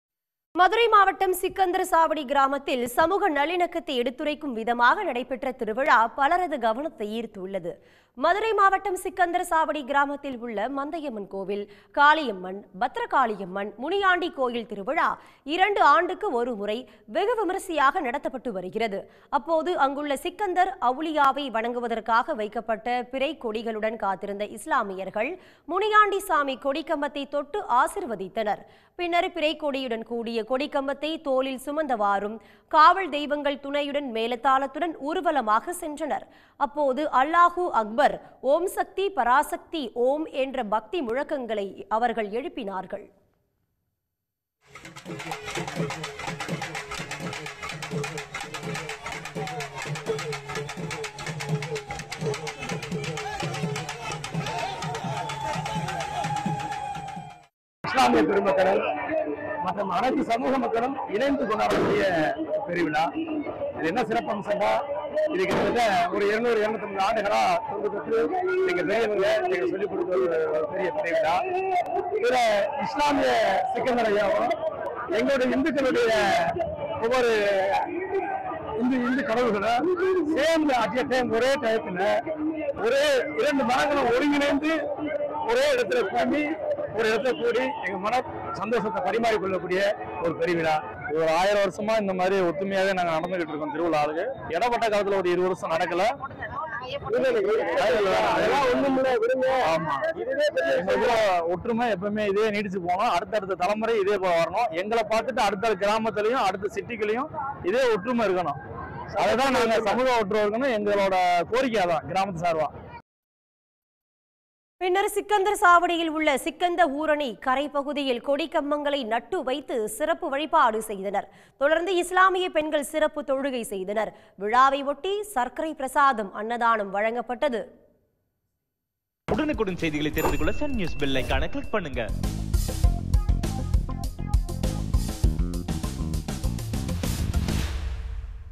மதுரையில் அனைவரது கவனத்தை ஈர்த்த வினோத திருவிழா -ஒரே நேரத்தில் ஒலித்த அல்லாஹ், ஓம் சக்தி முழக்கங்கள்
மதுரையில் அனைவரது கவனத்தை ஈர்த்த வினோத திருவிழா -ஒரே நேரத்தில் ஒலித்த அல்லாஹ், ஓம் சக்தி முழக்கங்கள் | Sunnews